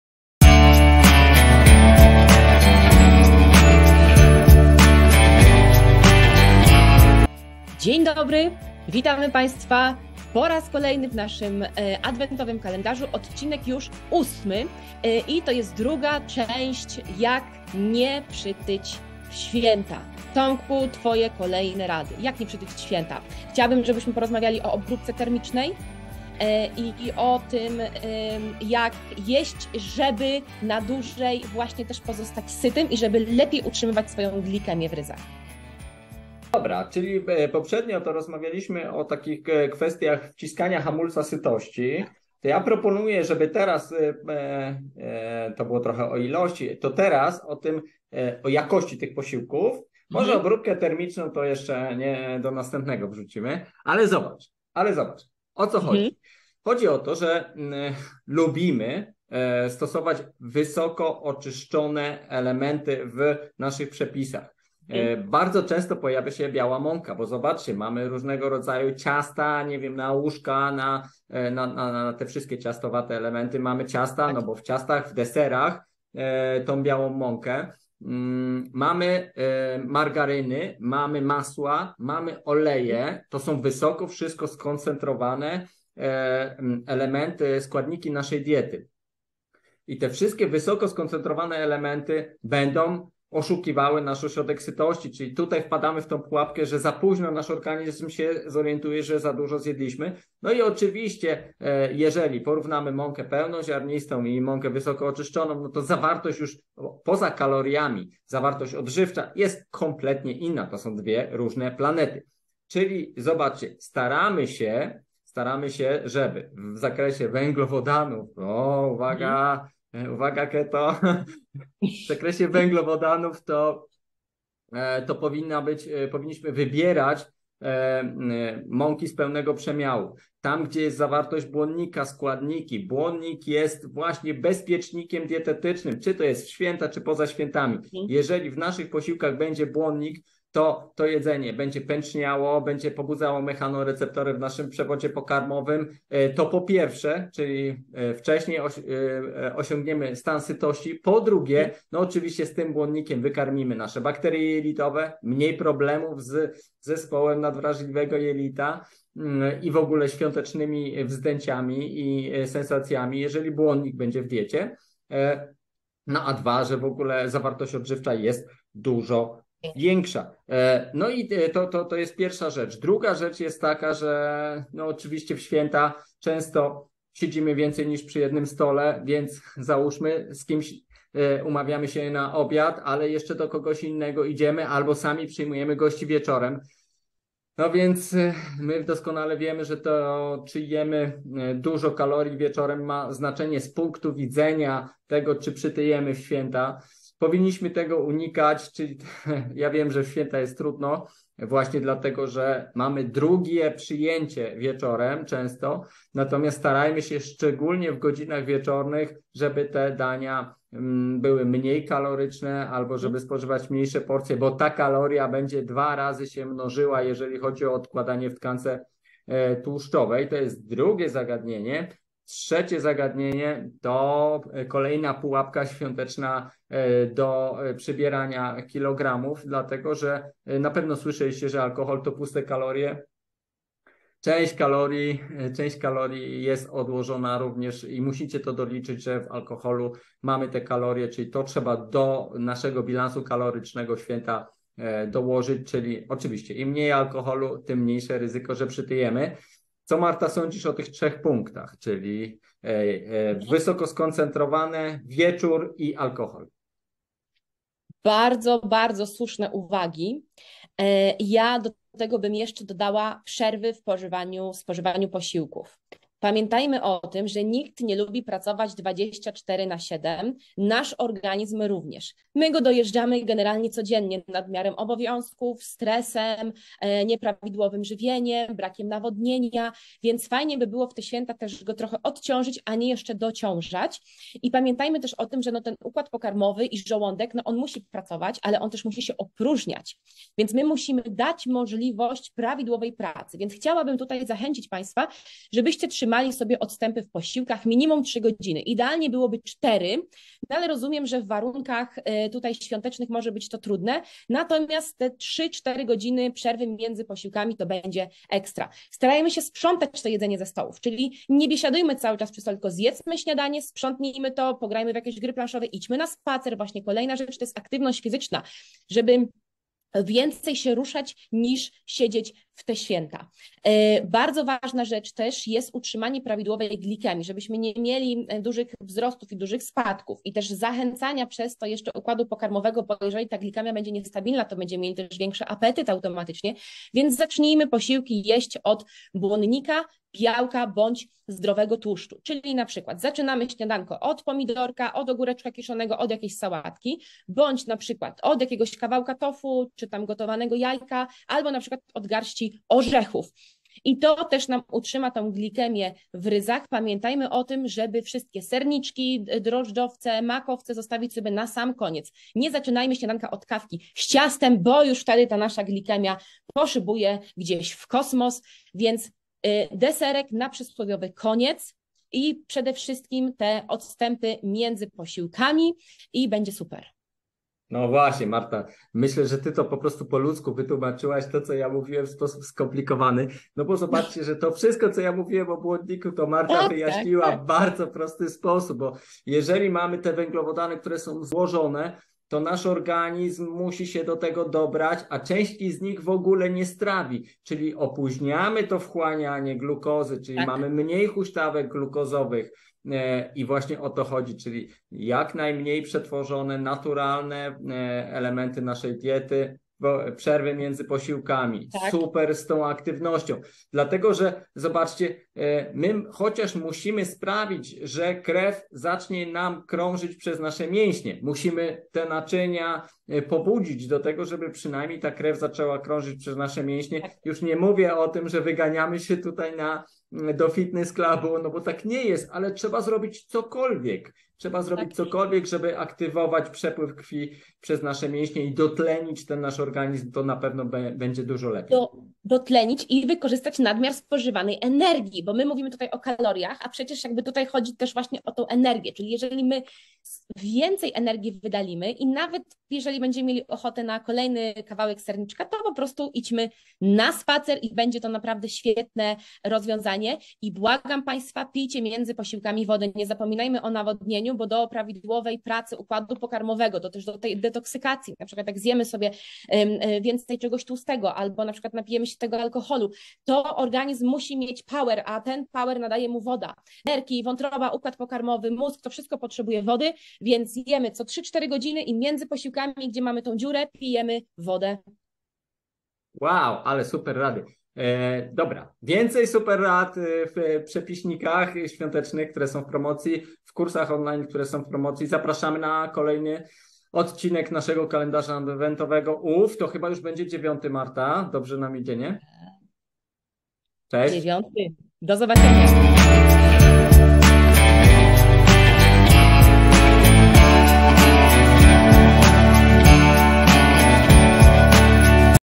Jak nie przytyć w Święta c.d ? Kalendarz Adwentowy cz. 8
OSTATNI DZWONEK NA PROMOCJE🎁💥😱
Jak nie przytyć w Święta? Ten temat okazał się bardzo… gruby i nie zmieścił się do jednego odcinka, więc w dzisiejszym nagraniu go kontynuujemy.
Mamy dla Was jeszcze więcej porad oraz ciekawostek. Marta Michalska-Wosztyl
Dowiecie się m.in.:
🎁Gdzie szukać możliwości odchudzenia przepisów?
🎁Co jest naszym bezpiecznikiem dietetycznym?
🎁Kiedy odpuścić najbardziej kaloryczne potrawy?
🎁Jakie odstępy między posiłkami powinniśmy zachować?